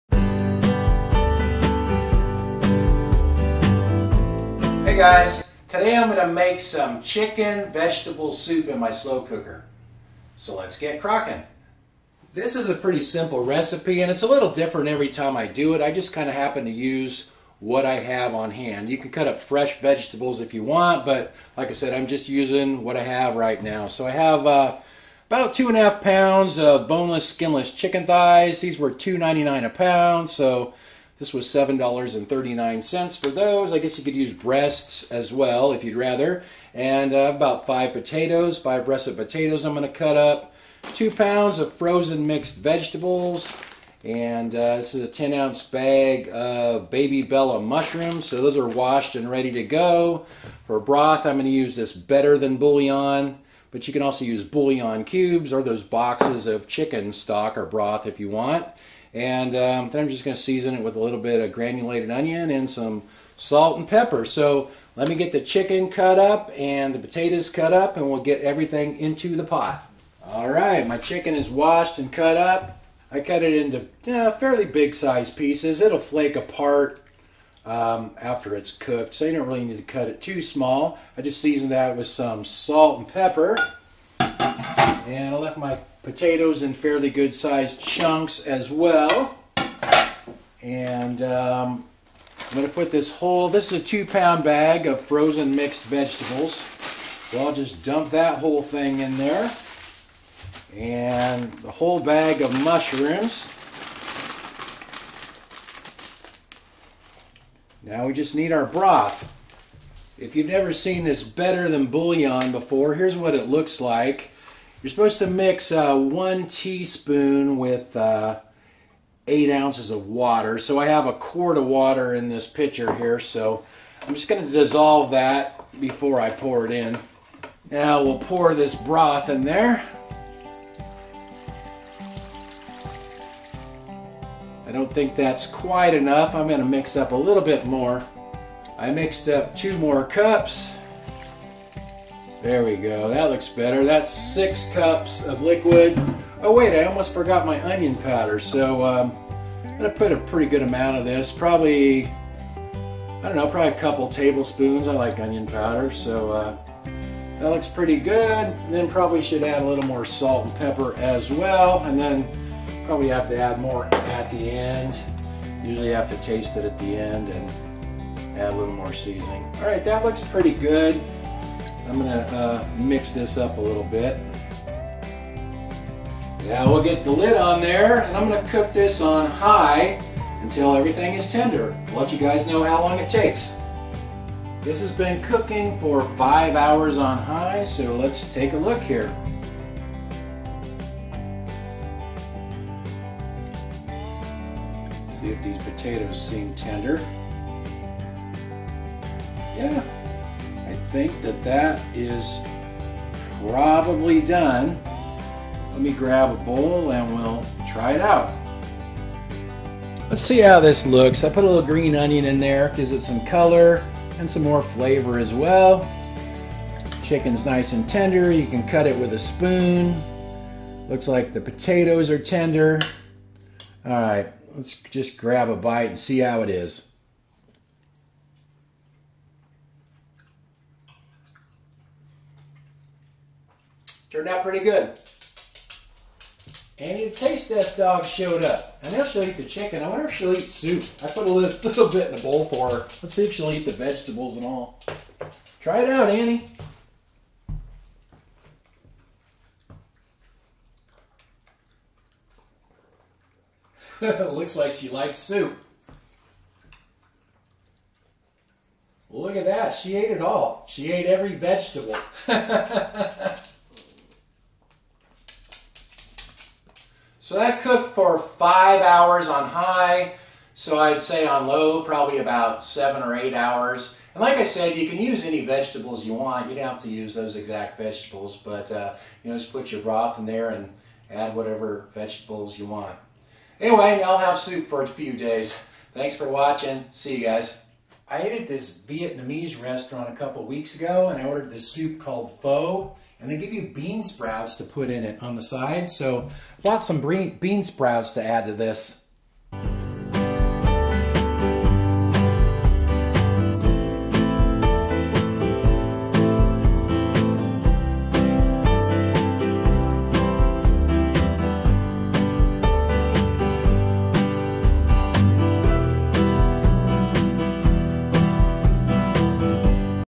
Simple Chicken Vegetable Soup in a Slow Cooker~Easy Cooking
In this easy cooking video, I make some chicken vegetable soup in my crockpot, or slow cooker. The ingredients I used are 2.5 lbs of boneless skinless chicken thighs, 5 potatoes, 10 oz baby bella mushrooms, 2 lbs of frozen mixed vegetables, 2 tablespoons onion powder, 6 cups of water, 6 teaspoons of Better Than Bouillon, and salt and pepper to taste. The cooking time is 5 hours on high or 8 hours on low.